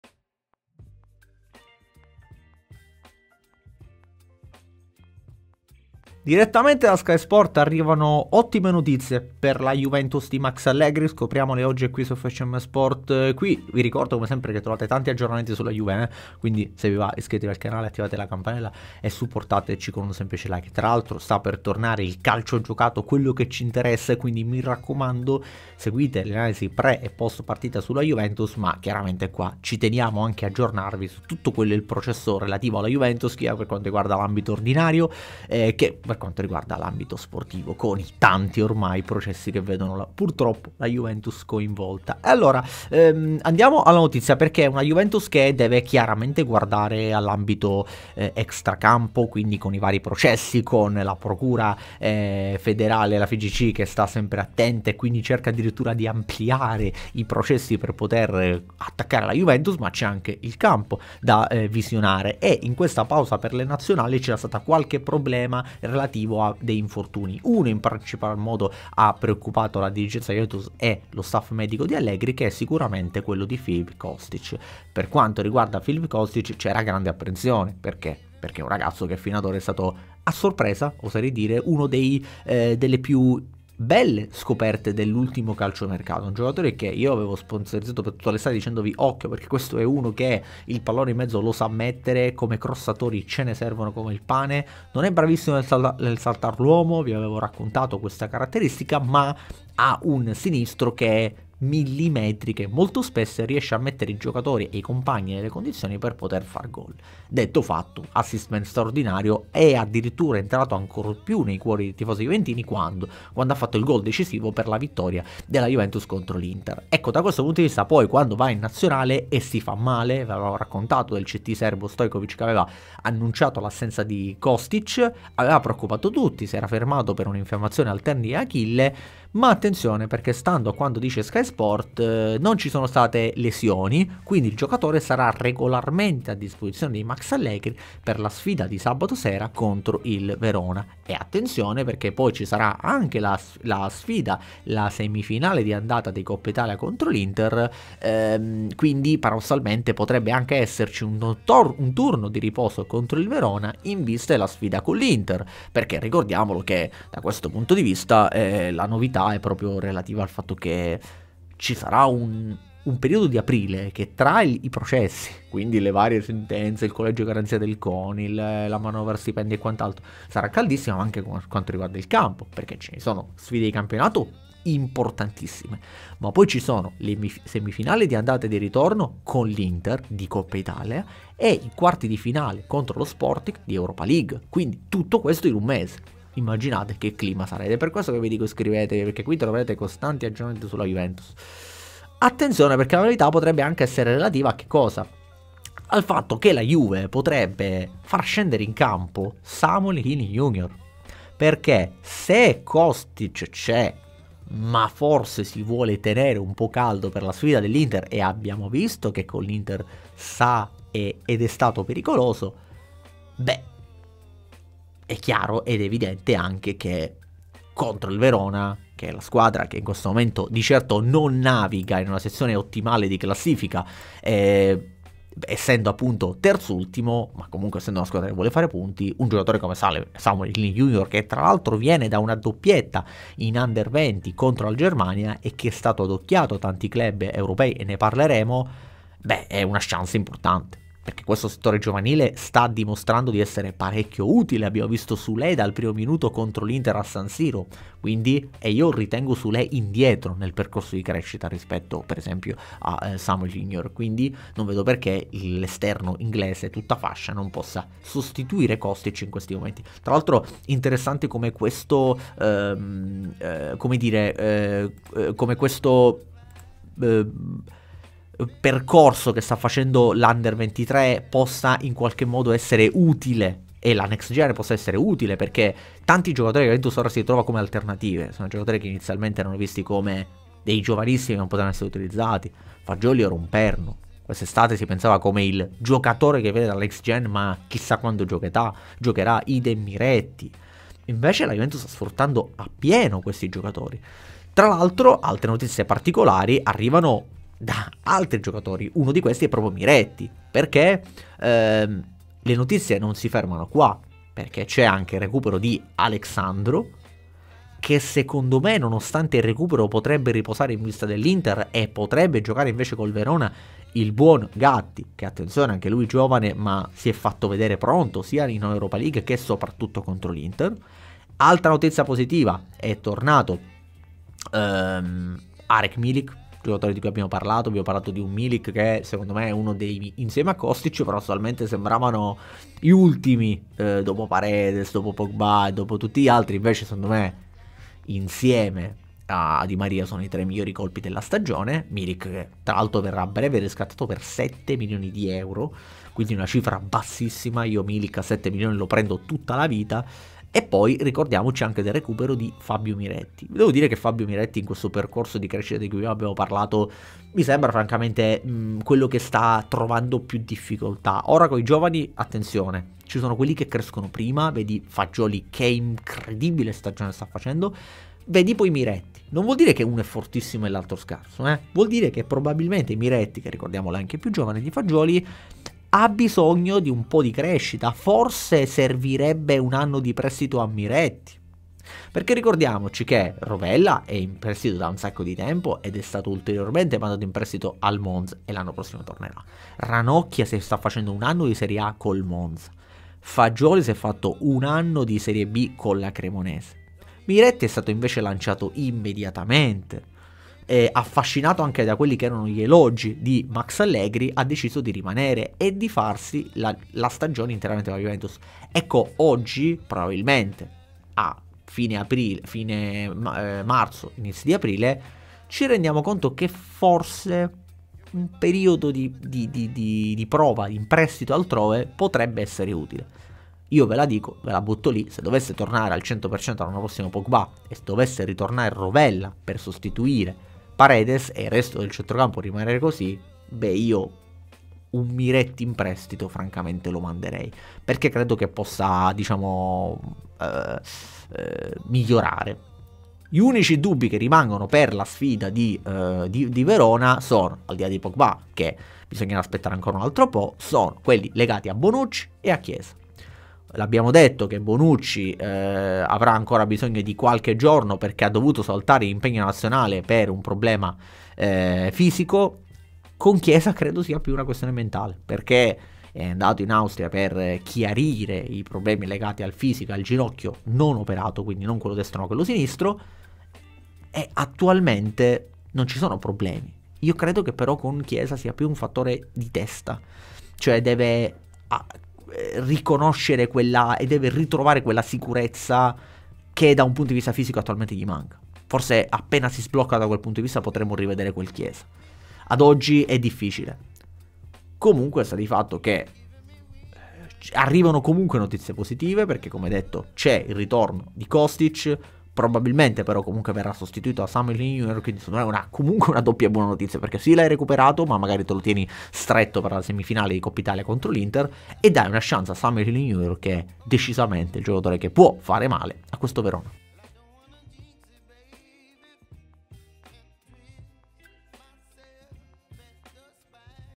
Juventus: recupera anche Kostic. Tutti disponibili contro il Verona? ||| Analisi Fcm Sport
SECONDO CANALE